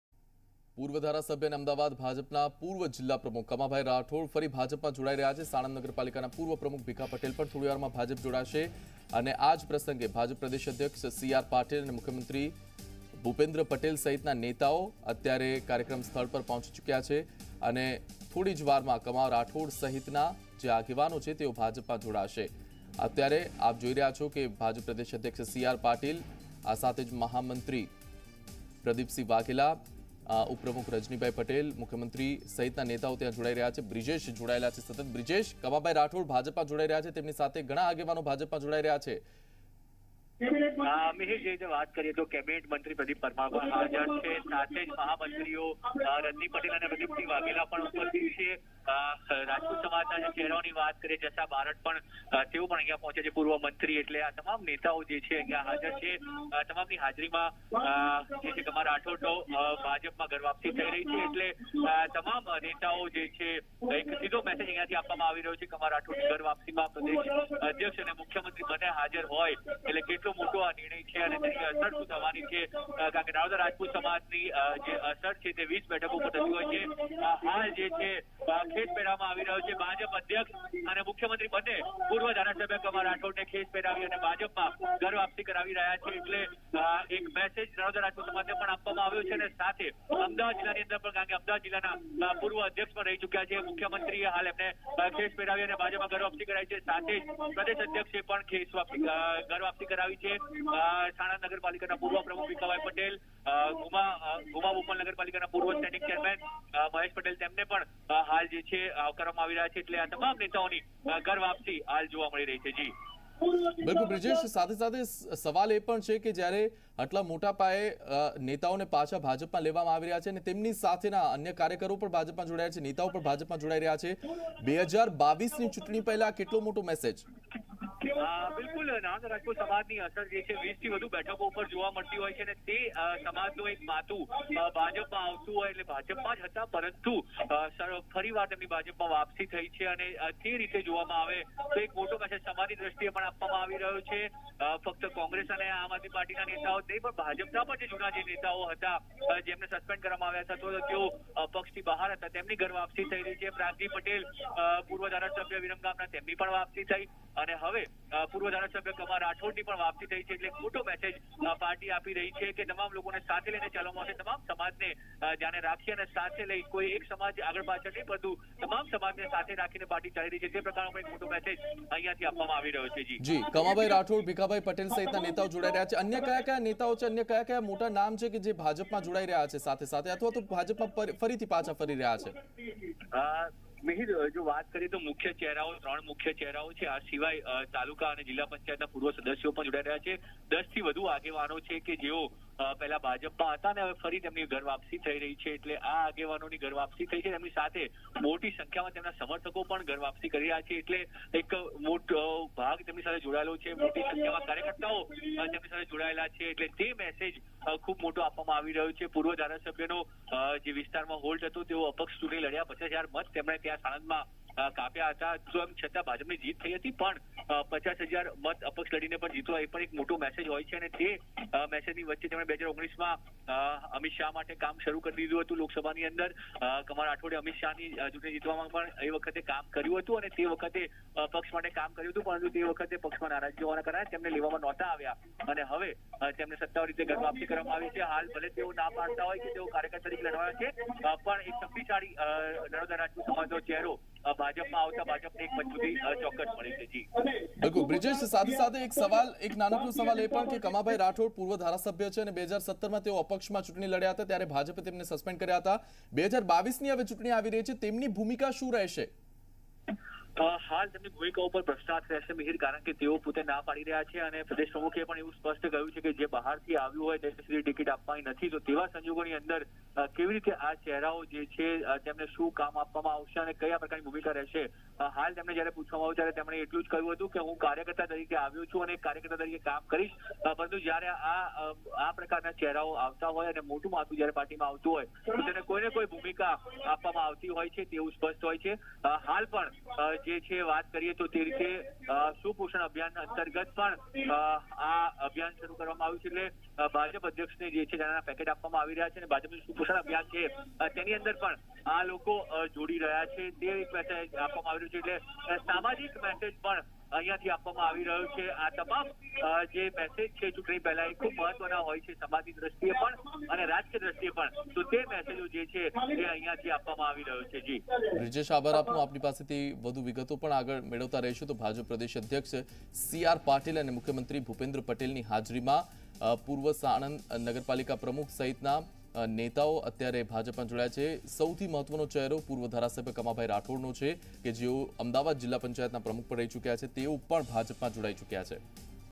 Suspended BJP leaders Kama Rathod, Bhikha Patel rejoin the party in presence of CR Paatil, CM Patel
Suspended BJP leaders Kama Rathod, Bhikha Patel rejoin the party in presence of CR Paatil, CM Patel | Zee News

ભાજપે સસ્પેન્ડ કરેલા નેતાઓની થોડીવારમાં ઘરવાપસી થશે--
પૂર્વ ધારાસભ્ય અને અમદાવાદ ભાજપના પૂર્વ જિલ્લા પ્રમુખ કમાભાઈ રાઠોડ ફરી ભાજપમાં જોડાશે--
સાણંદ નગરપાલિકાના પૂર્વ પ્રમુખ ભીખા પટેલ પણ થોડીવારમાં ભાજપમાં જોડાશે--
આ ઉપરાંત બોપલ ઘુમા પાલિકાના પૂર્વ સ્ટેન્ટિંગ કમિટી ચેરમેન પણ ભાજપમાં જોડાઈ રહ્યા છે--
ભાજપમાં જોડાતા પહેલા ઝુંડાલ સર્કલથી કમલમ સુધી રેલીનું પણ આયોજન કરાયું છે--
પ્રદેશ ભાજપ અધ્યક્ષ સીઆર પાટીલ અને મુખ્યમંત્રી ભૂપેન્દ્ર પટેલની અધ્યક્ષતામાં જોડાશે--
આજે ભાજપમાં જોડાનારા પૂર્વ ધારાસભ્ય કમા રાઠોડ 2007માં ધારાસભ્ય રહી ચૂક્યા છે અને નાડોદા સમાજનો મોટો ચહેરો છે... અગાઉ તેઓ ભાજપના અમદાવાદ જિલ્લા પ્રમુખ રહી ચૂક્યા છે અને 2017માં અપક્ષ ચૂંટણી લડ્યા એટલે ભાજપે તેમને સસ્પેન્ડ કર્યા હતા... જે બાદ 2019માં લોકસભા ચૂંટણી વખતે તેઓ પાછા ફર્યા  હતા ગૃહમંત્રી અમિત શાહ માટે કામ કર્યું હતું... હવે સત્તાવાર રીતે તેઓ ફરી ભાજપમાં ઘર વાપસી કરી રહ્યા છે